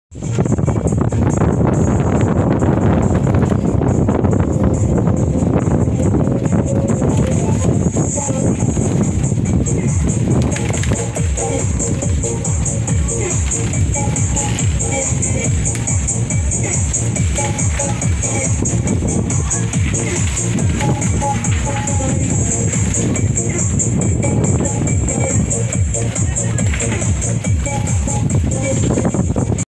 Sandbar in Escalante
This is us on a floating cottage at one of the Sandbars in Escalante, Philippines. As the you can see the weather is perfect and the water is clear